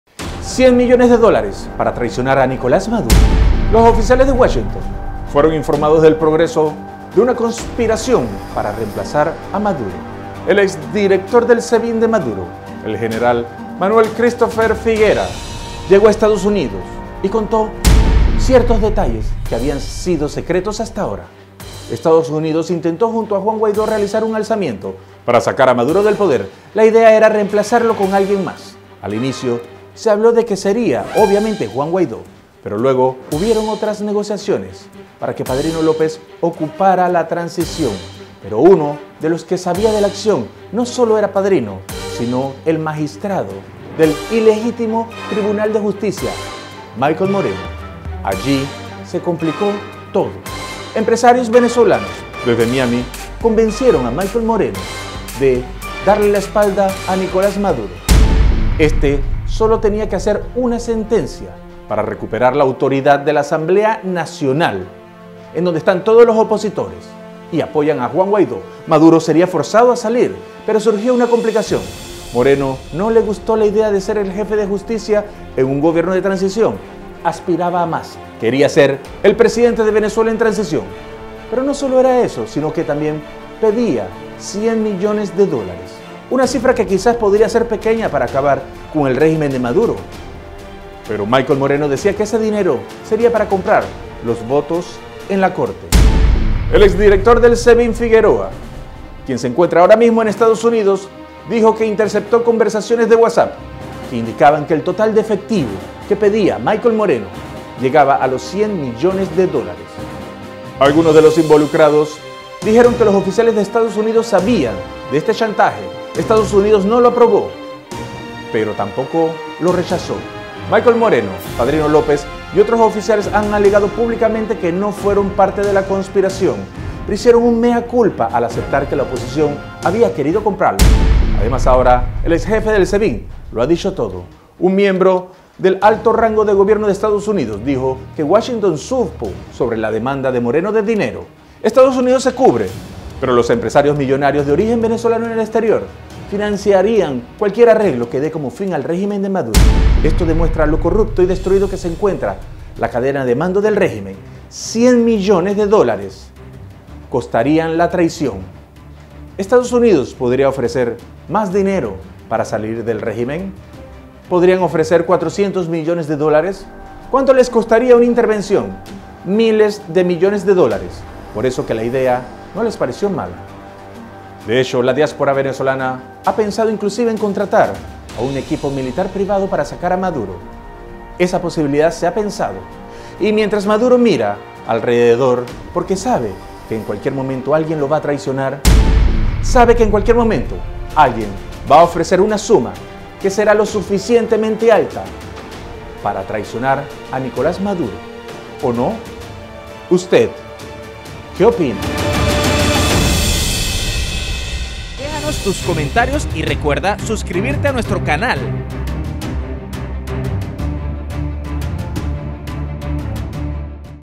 100 millones para traicionar a Maduro
Venezuela. 100 millones para traicionar a Maduro. 26 junio 2019. Cristopher Figuera dijo que ese era el precio para Maikel Moreno.

Este es un analisis de opinion sobre la situacion de Venezuela y su relacion con EEUU y los paises de la region.

Figuera by Figuera twitter own work
Figuera y Maduro by Prensa Presidencial
Foto de Padrino Lopez Attribution Ricardo Patiño
Maduro y Mikel by Presidencia El Salvador
Mikel Moreno By Government of Venezuela
Padrino Lopez by Prensa Presidencial
Padrino parado PNG by Ministério da Defesa Brasil